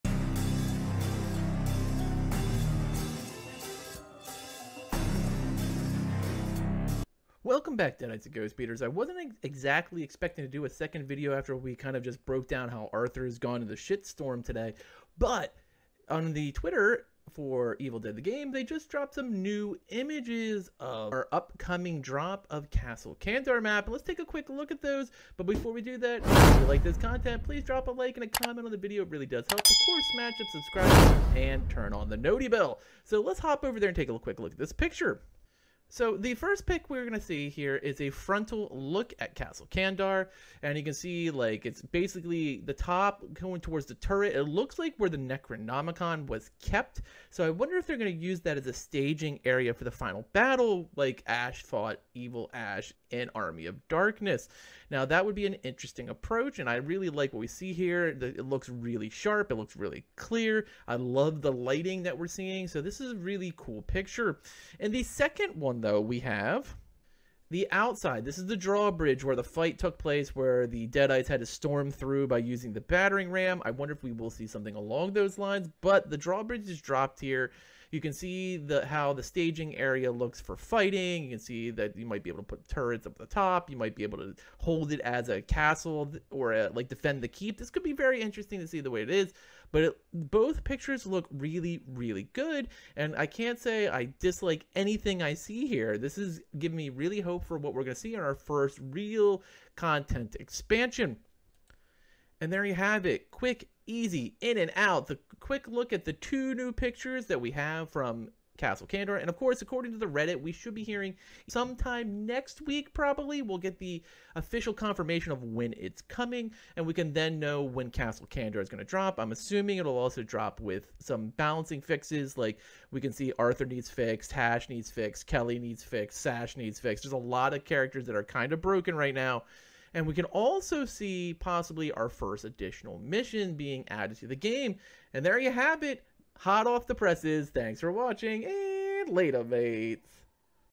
New Castle Kandar images dropped by Saber for Evil Dead: the Game
We get two more pics to look at the upcoming Map drop for Castle Kandar